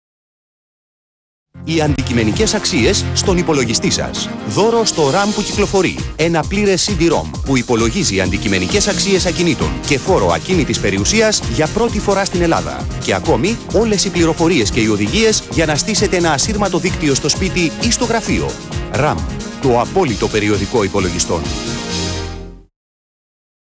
ΔΙΑΦΗΜΙΣΗ RAM 2003
Διαφήμιση περιοδικού Ram στην Ελληνική τηλεόραση το 2003